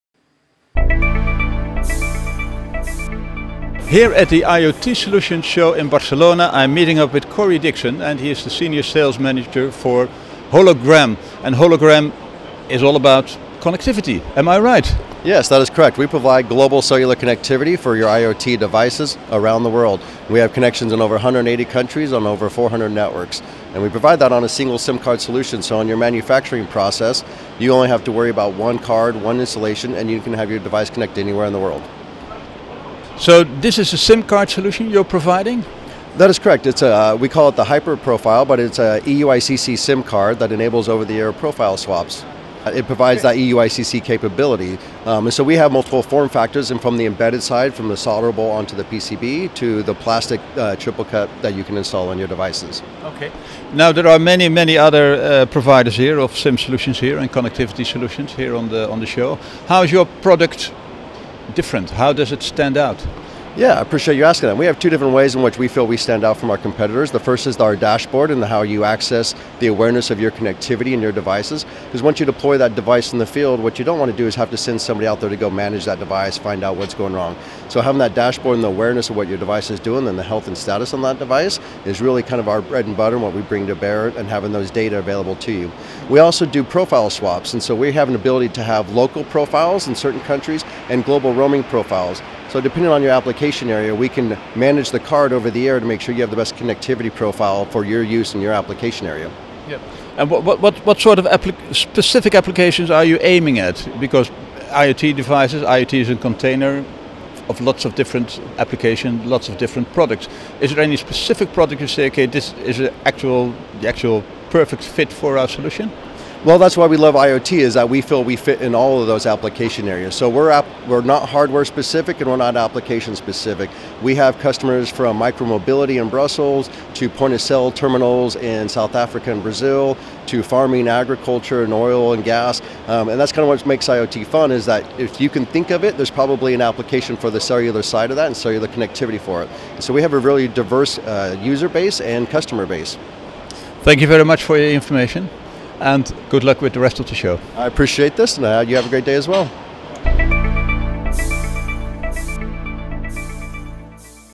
eeNews Europe Interview with Hologram, Cory Dixon
At the IoT Solutions Show in Barcelona (2023) eeNews is meeting up with Hologram to discuss IoT connectivity